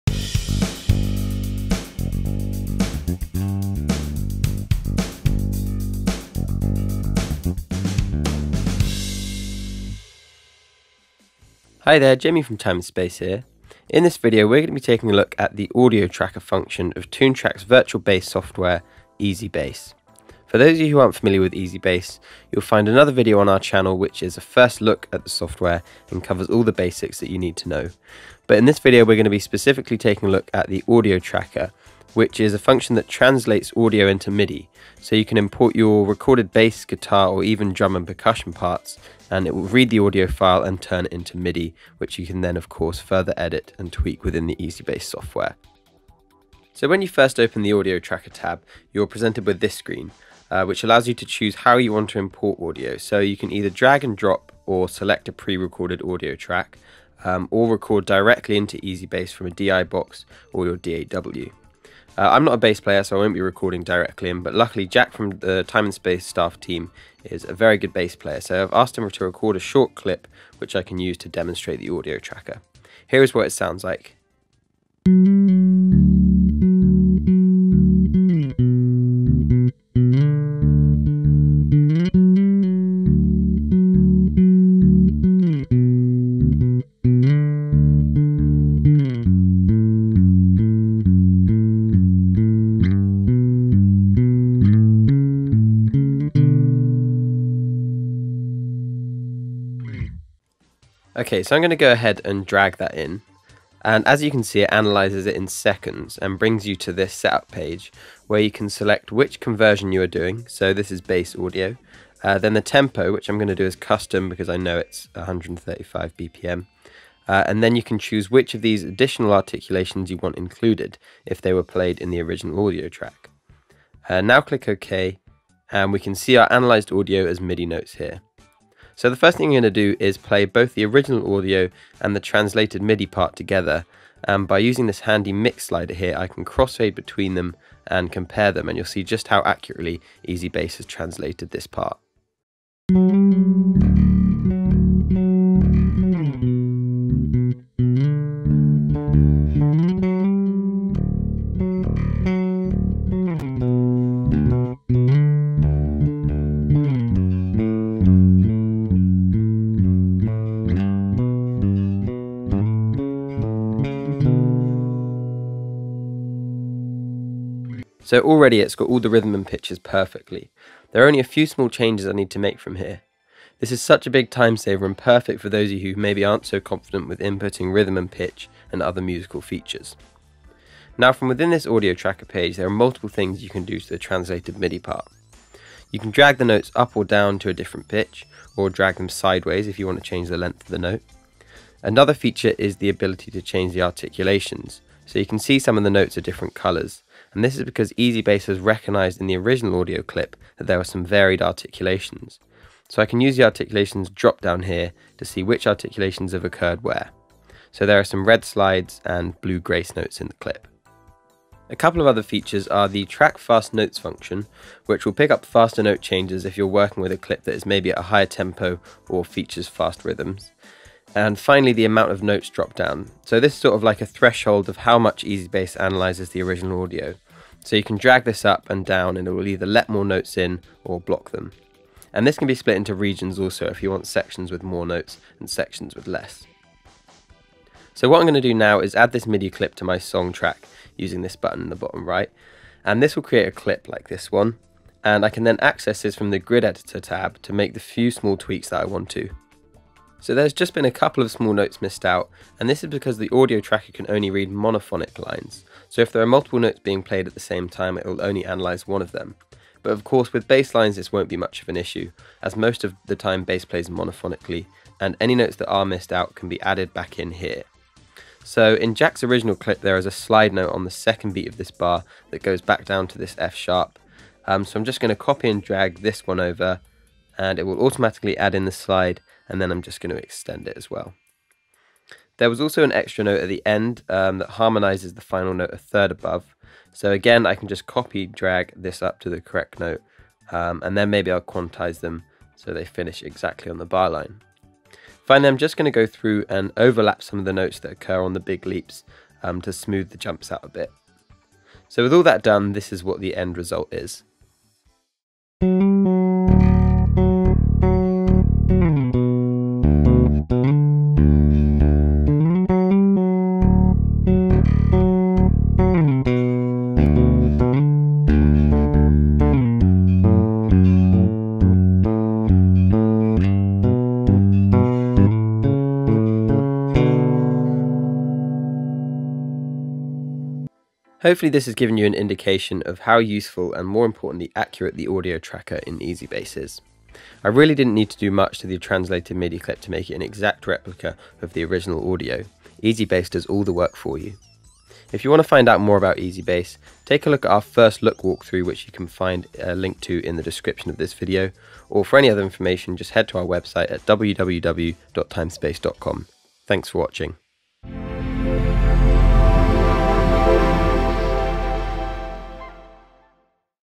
Toontrack EZbass bass guitar software plugin - The Audio Tracker
In this video, we take a look at the Audio Tracker tab of Toontrack's awesome virtual bass guitar software EZBass.

Turn anything into a bass. Well, almost at least! The ‘Audio Tracker’ was designed to mainly handle monophonic guitar or bass audio, but it also has a deep understanding for rhythmic material. In effect, this means you can use an already recorded guitar, bass or percussive performance, import it to EZbass and have it seamlessly ‘translate’ it to MIDI that you can continue to perfect using the tools available in the software. You can even record your own audio, right inside EZbass!

Toontrack EZbass is the first bass virtual instrument of its kind with a pristine sound and fundamental features for effortlessly adding bass guitar to your songs. In short, buy EZbass and you'll be making killer bass guitar tracks in seconds!